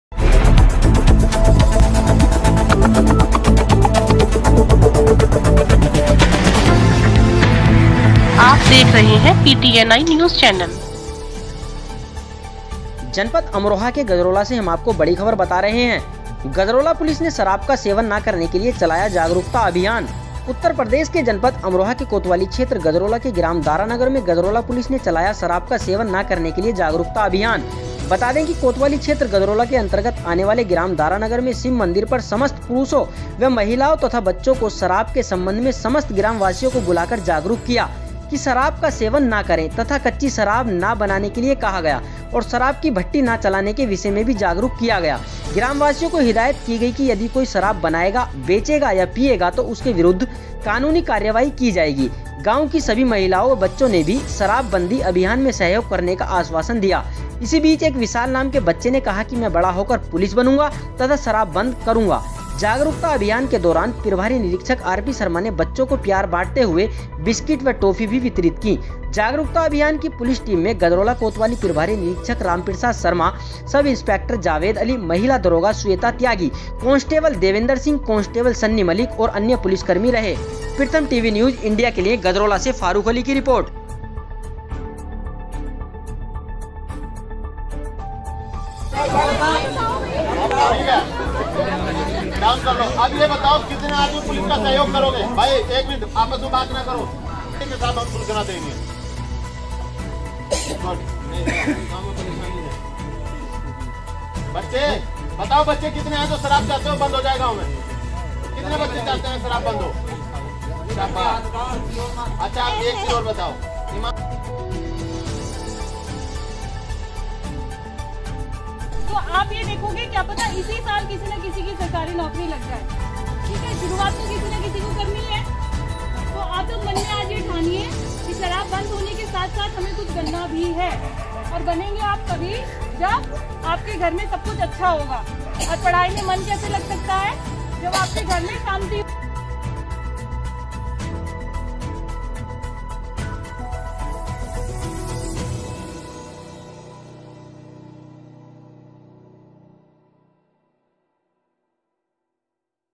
गजरौला पुलिस के SHO ने शराब का सेवन ना करने के लिए चलाया जागरूकता अभियान -PTNI
कोतवाली क्षेत्र गजरौला के अंतर्गत आने वाले ग्राम दारानगर में शिव मंदिर पर समस्त पुरुषों व महिलाओं तथा बच्चों को शराब के संबंध में समस्त ग्राम वासियों को बुलाकर जागरूक किया कि शराब का सेवन ना करे।तथा कच्ची शराब ना बनाने के लिए कहा गया और शराब की भट्टी ना चलाने के विषय मे भी जागरूक किया गया। ग्राम वासियों को हिदायत की गई कि यदि कोई शराब बनाएगा,बेचेगा या पियेगा तो उसके विरूद्ध कानूनी कार्यवाही की जाएगी।गांव की सभी महिलाओं व बच्चों ने भी शराब बंदी अभियान में सहयोग करने का आश्वाशन दिया। इसी बीच एक विशाल नाम के बच्चे ने कहा कि में बड़ा होकर पुलिस बनूंगा तथा शराब बंद करूंगा।
जागरूकता अभियान के दौरान प्रभारी निरीक्षक आरपी शर्मा ने बच्चों को प्यार बांटते हुए बिस्किट व टॉफी भी वितरित की।
जागरूकता अभियान की पुलिस टीम में गजरौला कोतवाली प्रभारी निरीक्षक रामप्रसाद शर्मा, सब इंस्पेक्टर जावेद अली ,महिला दरोगा श्वेता त्यागी, कांस्टेबल देवेंद्र सिंह ,कॉन्स्टेबल सन्नी मलिक और अन्य पुलिसकर्मी रहे।
प्रथम टीवी न्यूज़ इंडिया के लिए गजरौला से फारूख अली की रिपोर्ट